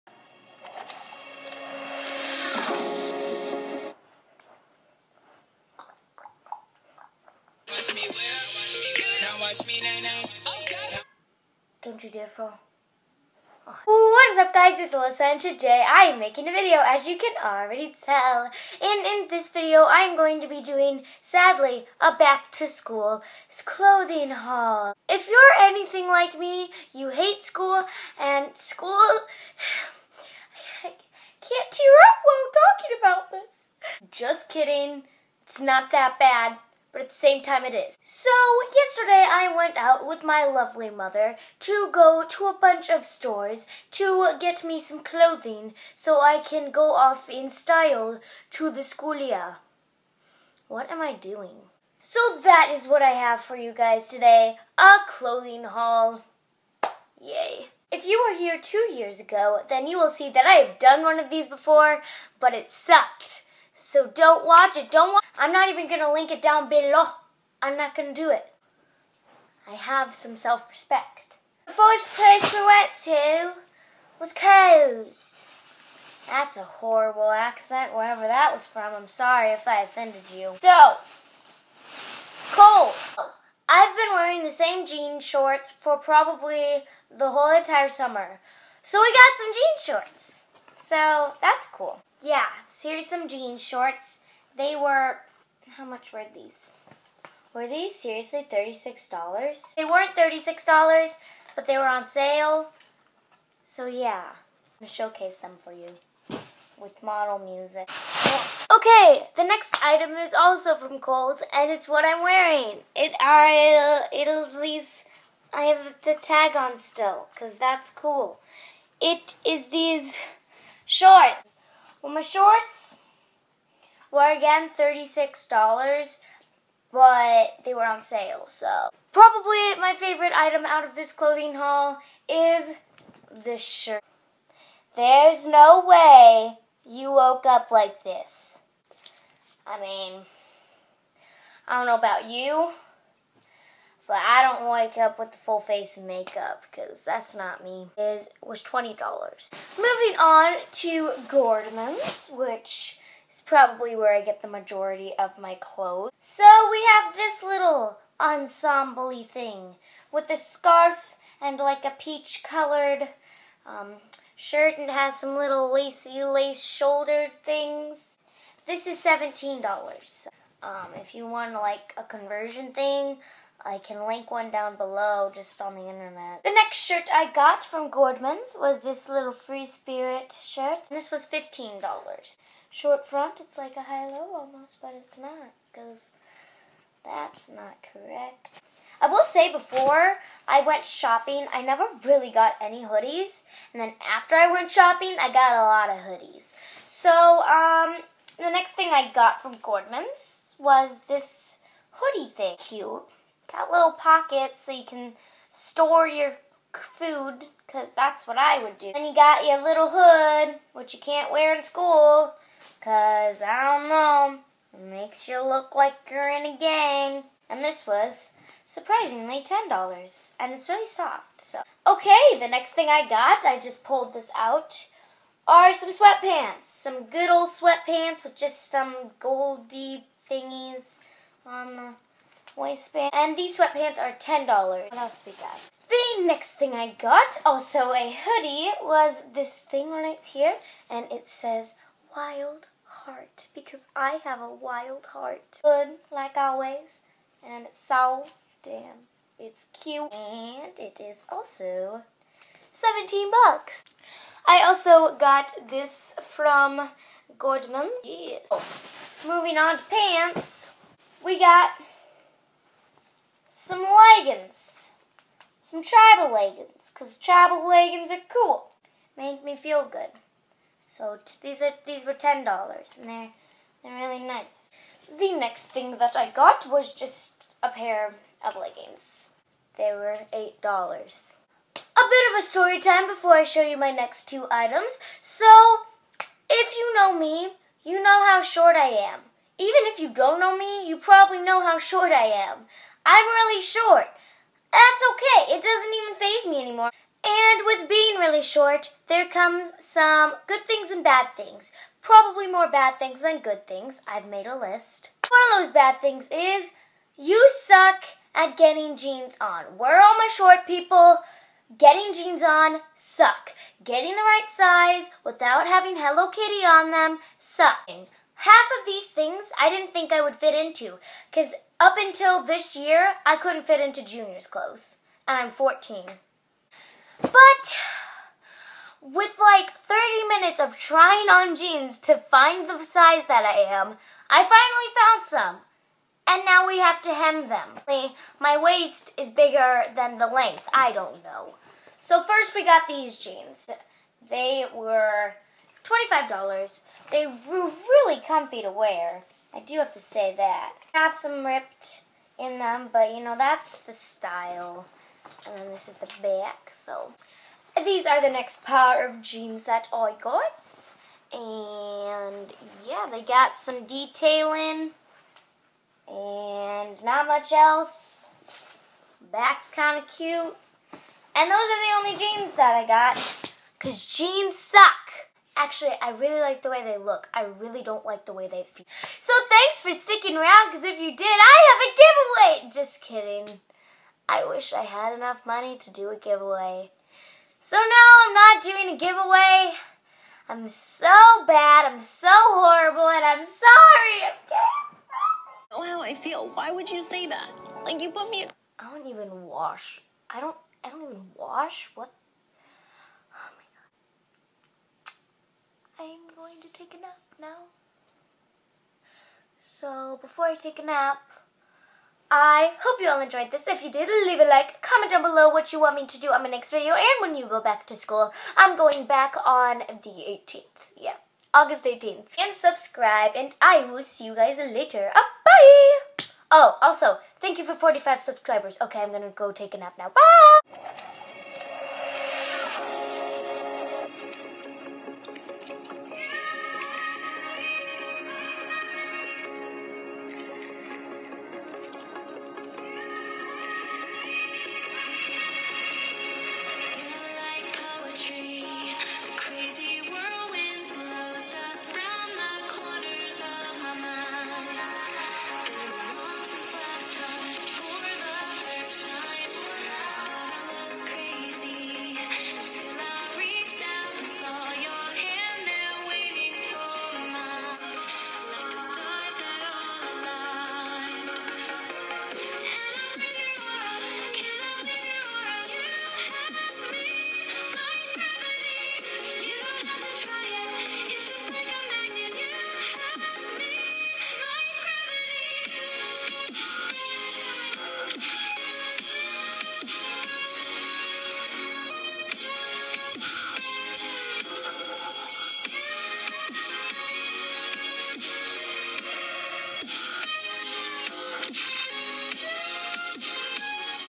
Back To School Clothing Haul
SEE I DIDNT LINK IT HA. So I'm now in school, this was a while ago.

I created some Sim challenges! Check them out and maybe even try them here: 1.

All clothing items you see are from Kohl's and Gordmans

Comments are always accepted... Unless they're naughty, mean, or hurtful... Keep em' clean!